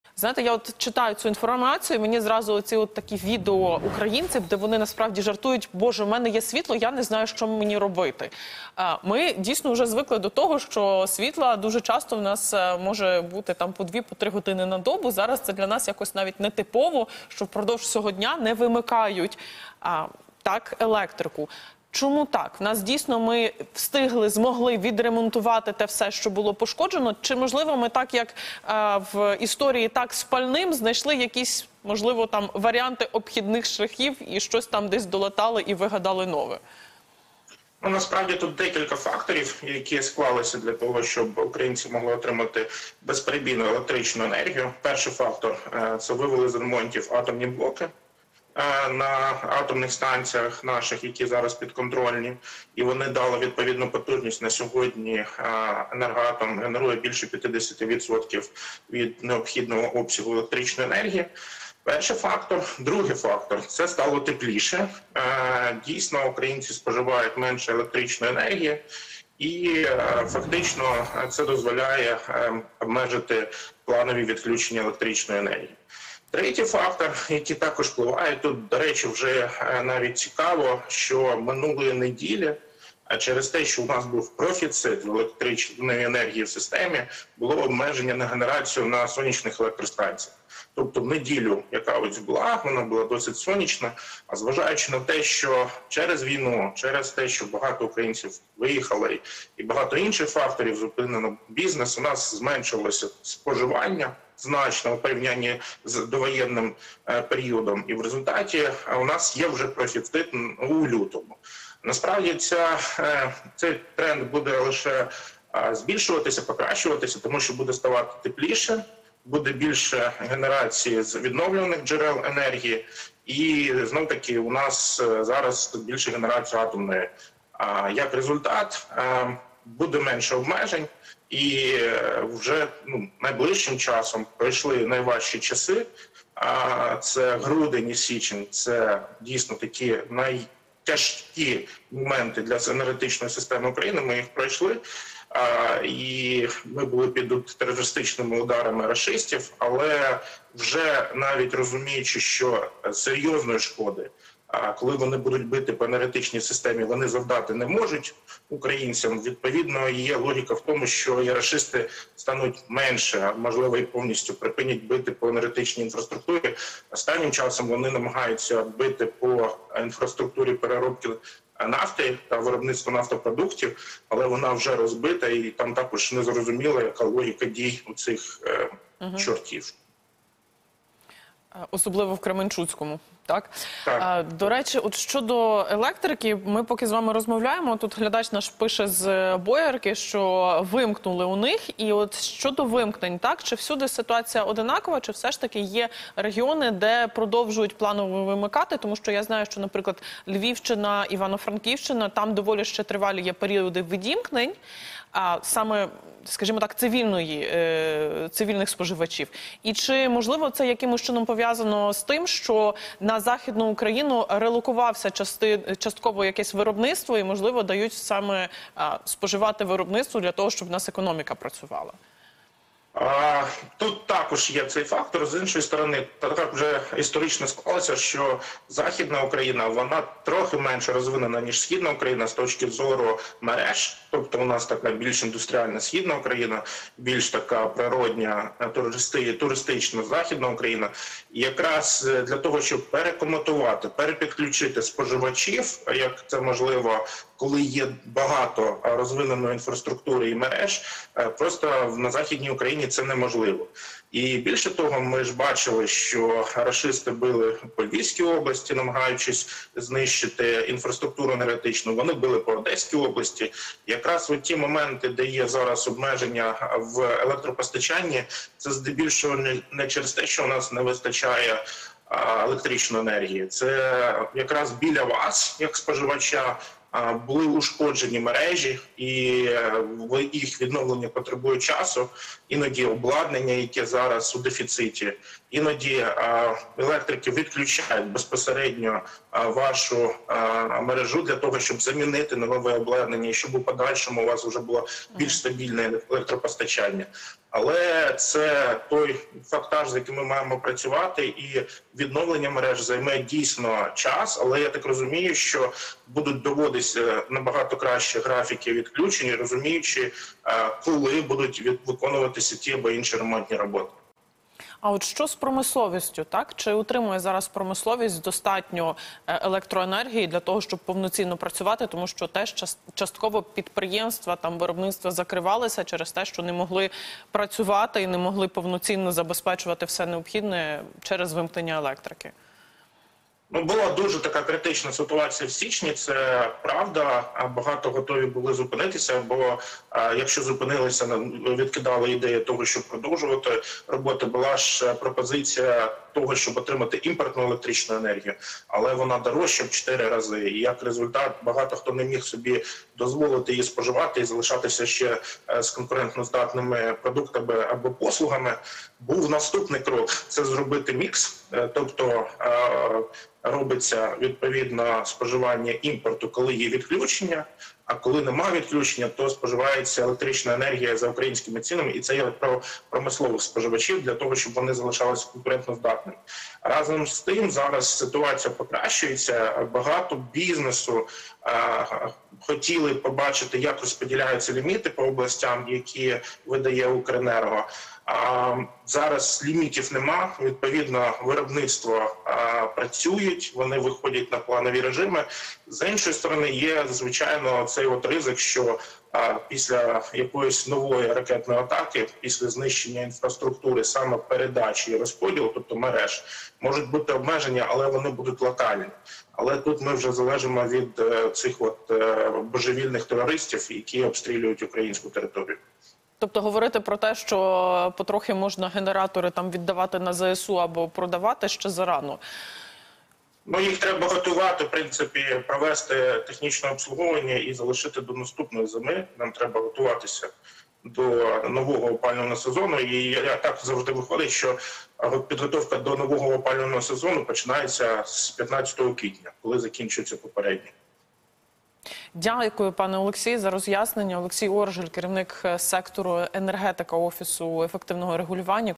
Енергосистема України пройшла найважчий період - далі буде легше - Олексій Оржель
Олексій Оржель, керівник сектору «Енергетика» Офісу Ефективного Регулювання, міністр енергетики та захисту довкілля України у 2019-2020 рр в ефірі телеканалу Київ.